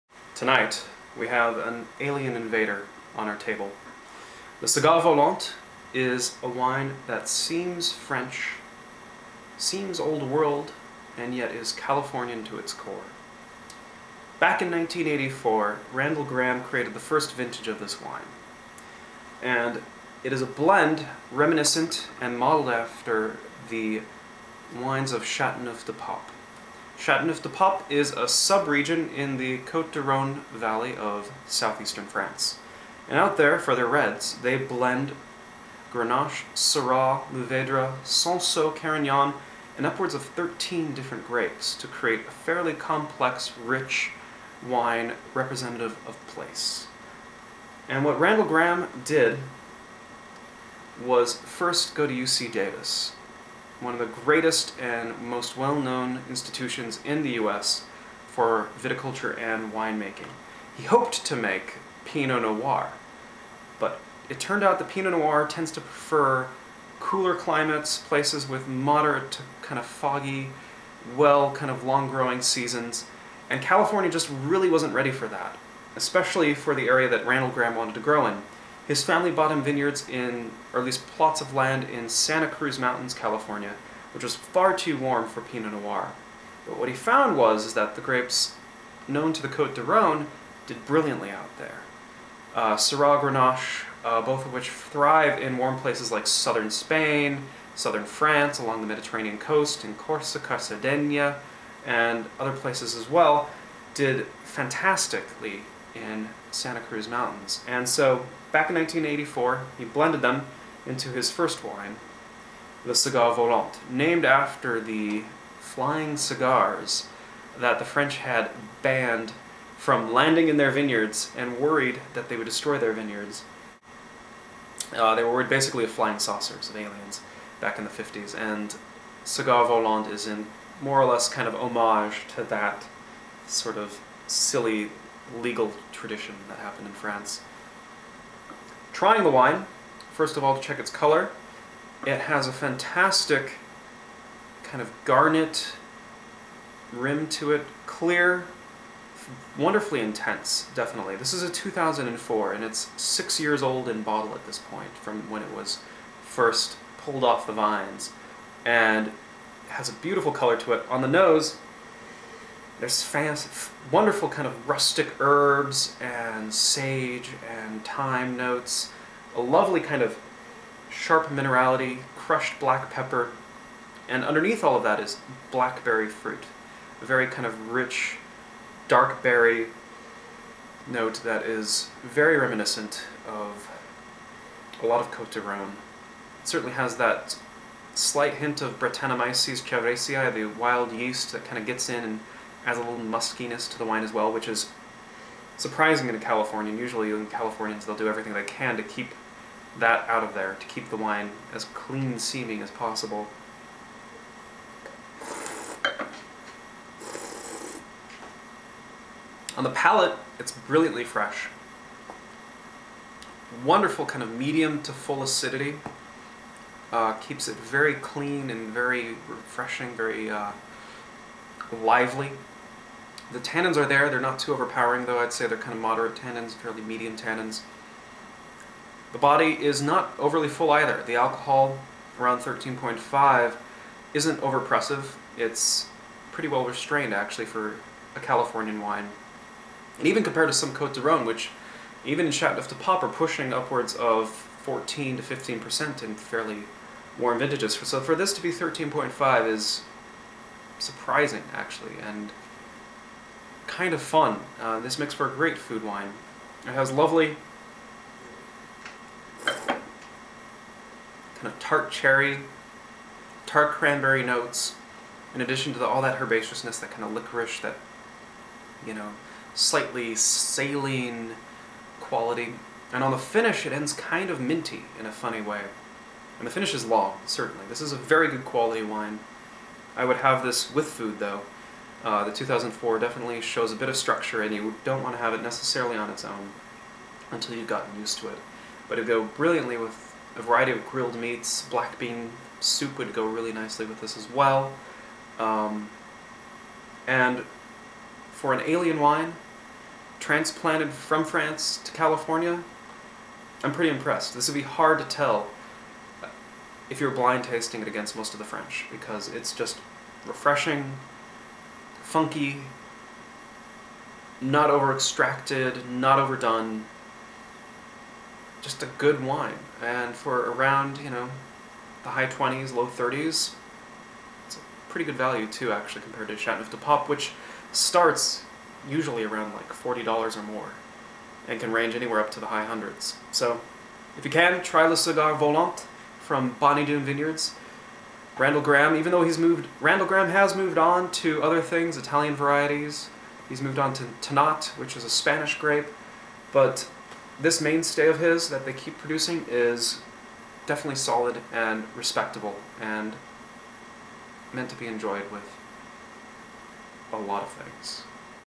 LeCigarVolant2004
My review of Bonny Doon's le Cigare Volant 2004 chateauneuf du pape red blend.